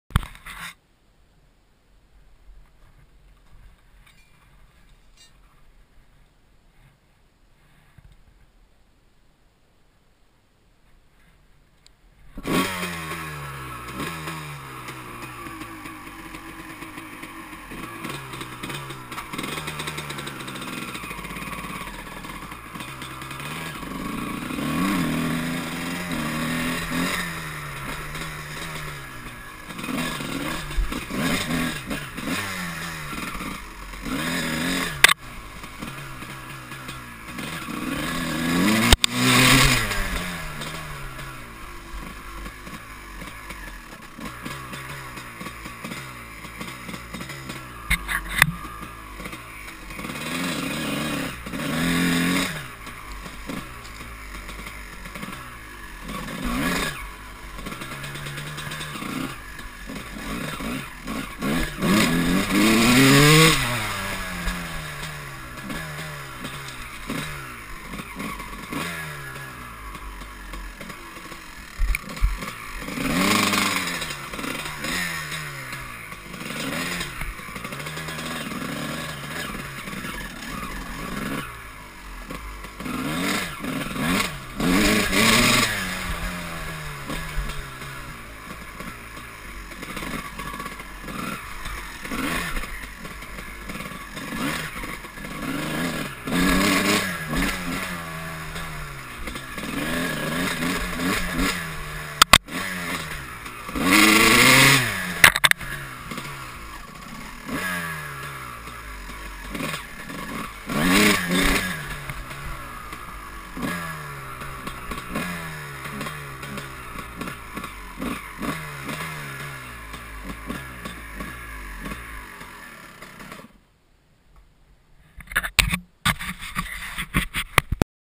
02 honda cr250 go pro hero test
testing my new go pro hero on my 02 cr250 through some trails near my house